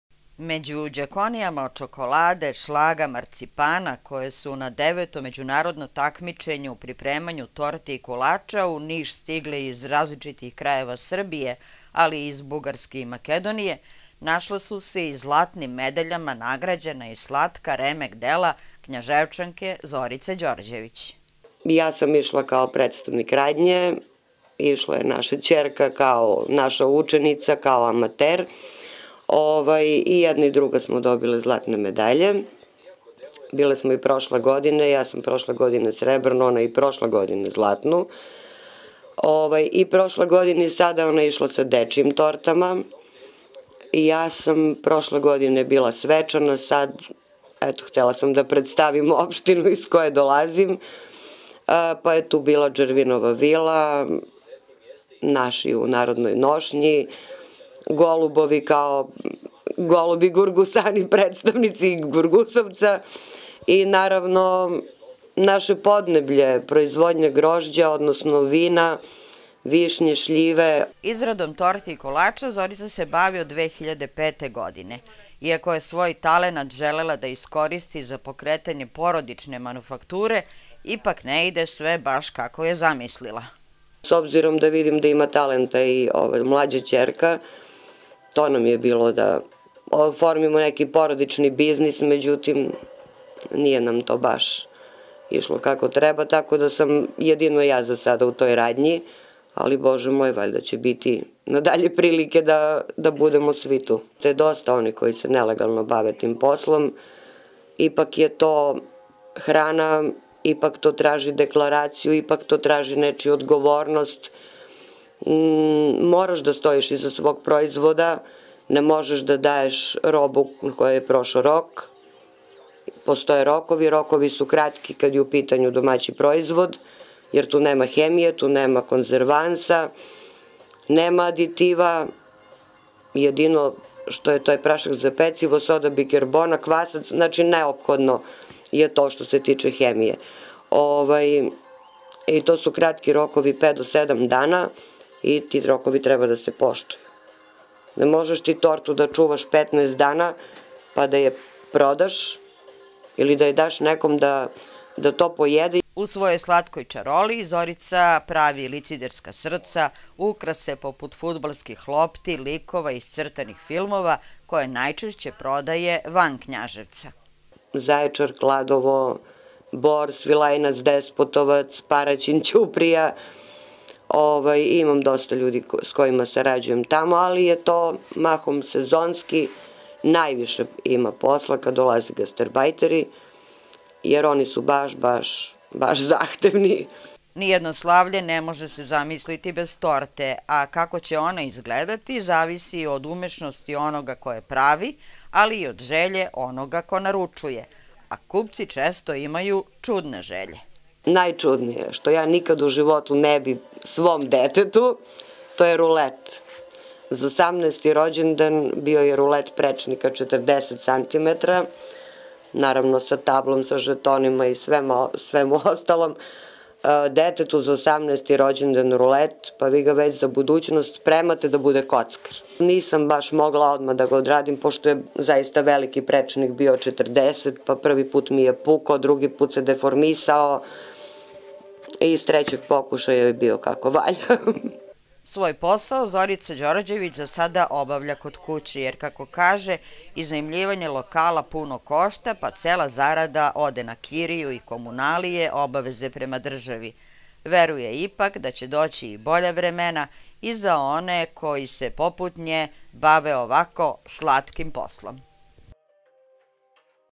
Zoričina slatka čarolija
Na Devetom medjunarodnom takmičenju u pripremanju torti i kolača, u organizaciji Udruženja ugostiteljskih i turistikih poslenika Niša, Knjaževčanka Zorica Đorđević osvojila je dve zlatne medalje za torte, nastale u njenoj radionici ''Slatka čarolija''.